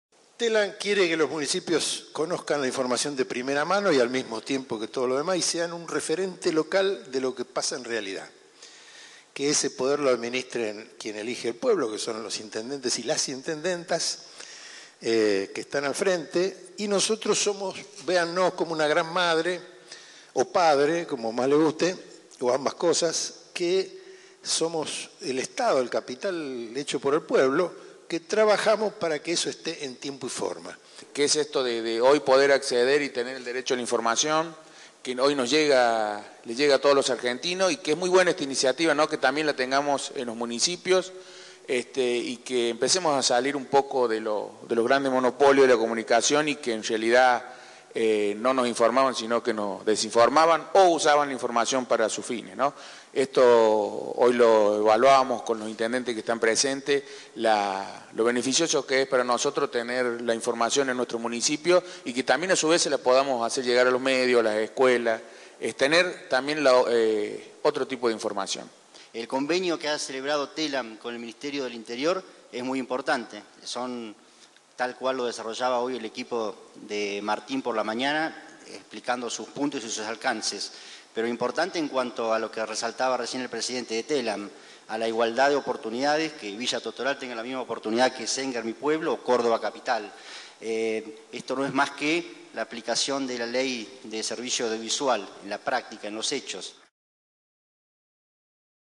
Télam firmó convenios con medios y municipios cordobeses
La agencia de noticias Télam firmó convenios con varios municipios y medios del norte cordobés para la provisión del servicio de noticias en sus distintos formatos, con el costo bonificado por un año.
Fue en el marco de las segundas jornadas de la Mesa de Gestión y Trabajo Regional, organizadas por Télam y la Subsecretaría de Gestión Municipal del Ministerio del Interior.
El encuentro se realizó en el Plaza Hotel de la localidad de Villa el Totoral, ubicado en el departamento Totoral,  a unos 81 kilómetros de la capital provincial.
Estuvieron presentes el presidente de Télam, Martín García; el jefe de Gabinete de la Subsecretaría de Gestión Municipal, Fabricio Casarosa; y el intendente local, Ernesto Bernabey; entre otras.